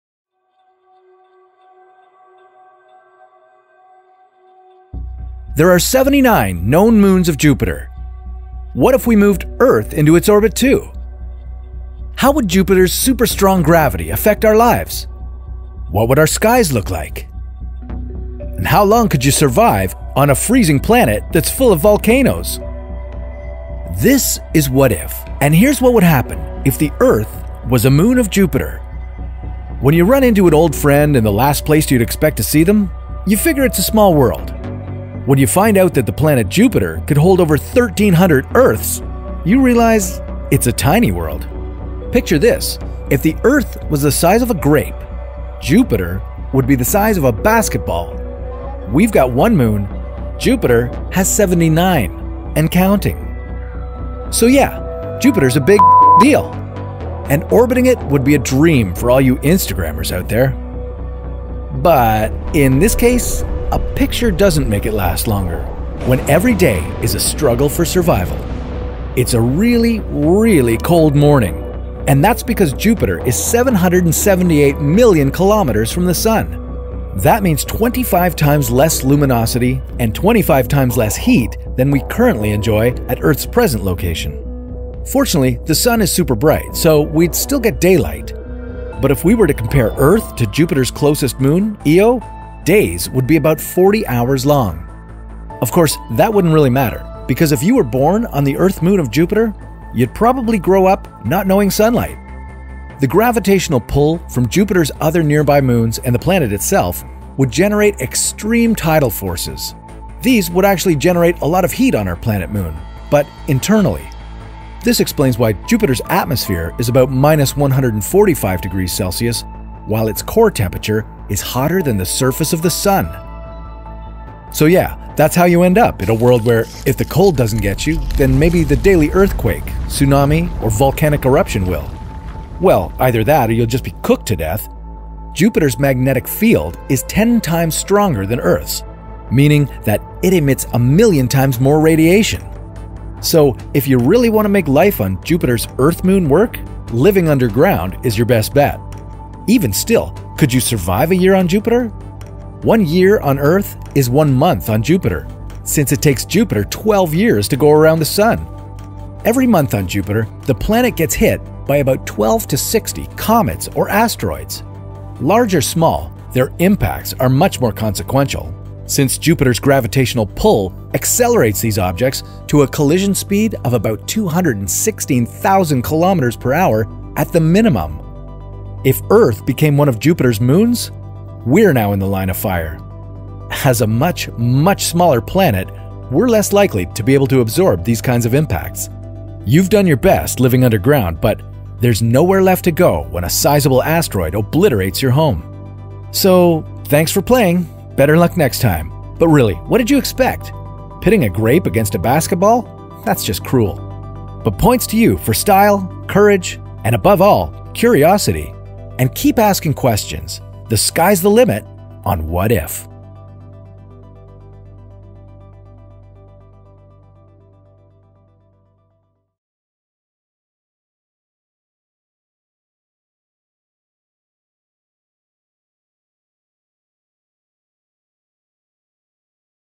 如果地球是木星的衛星, 木星超強的引力會影響我們的生活？
我們會在地下生活嗎？